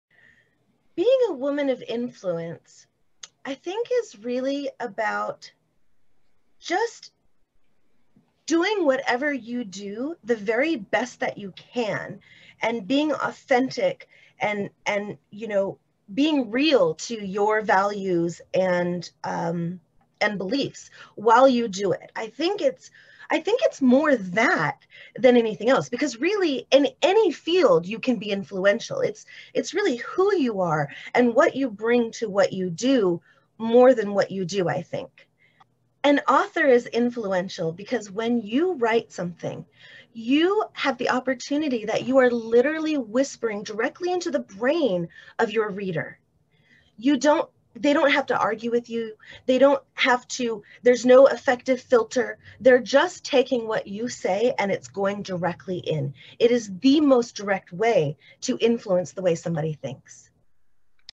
Author of Influence Finalist | Sandi Masori
Join us in celebrating one of our FABULOUS San Diego Women of Influence Awards Finalists, Sandi Masori! When circumstances shift, so do we - to our first ever WOIA LIVE SIMULCAST!
November 4, 2020 11:00 am - 1:00pm